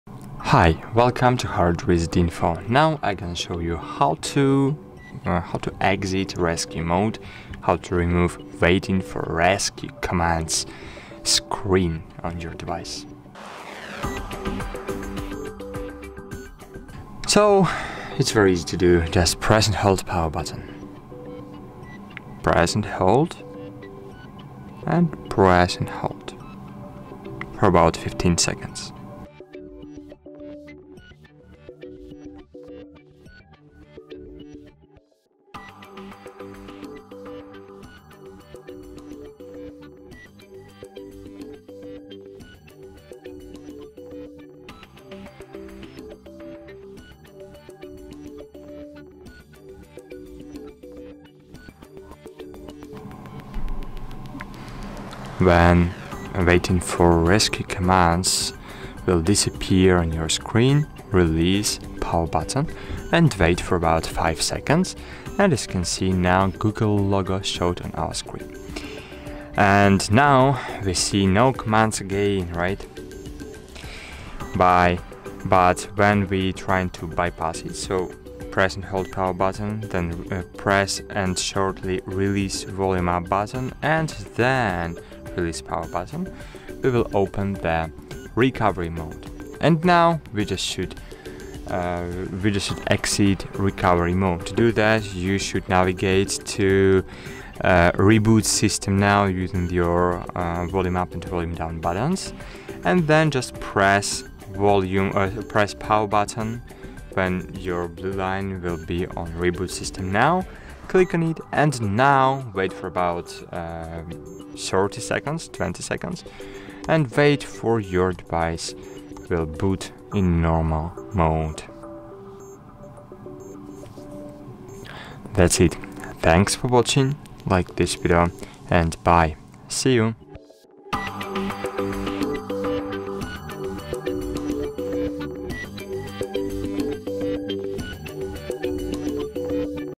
How to Exit Rescue Mode on Google Pixel 9
This tutorial will guide you through the steps to exit Rescue Mode on your Google Pixel 9. Rescue Mode is a useful feature for troubleshooting, but once you're done, you’ll want to safely exit it and return to normal operation. Follow our simple instructions to get your Pixel 9 back to its standard mode in no time.

What is Rescue Mode on the Pixel 9?
How do I exit Rescue Mode on the Pixel 9?
What should I do if my Pixel 9 is stuck in Rescue Mode?
Is there any risk of data loss when exiting Rescue Mode?
Can I reboot the Pixel 9 to exit Rescue Mode?